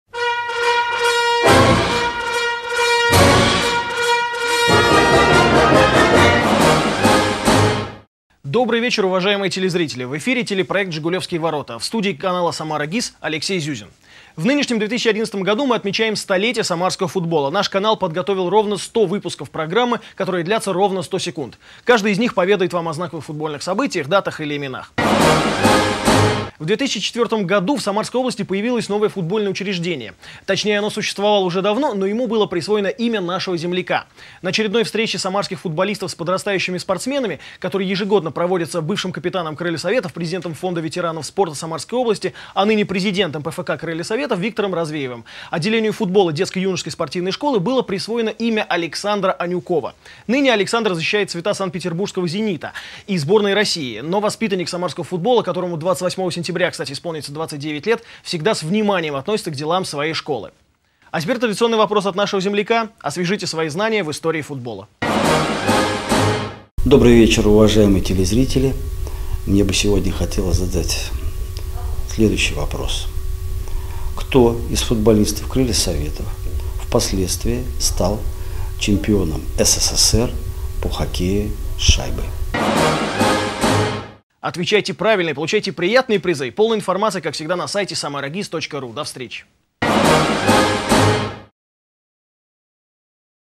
#89 Жигулевские ворота
В 2011 году исполняется 100 лет самарскому футболу. Первый матч в Самаре состоялся 11 сентября 1911 года на поле Вокзальной площади. Тот матч, между яхтсменами и кооператорами, практически не оставил о себе памяти. Но за век, отсчет которому он дал, событий на футбольных полях Самары накопилось великое множество.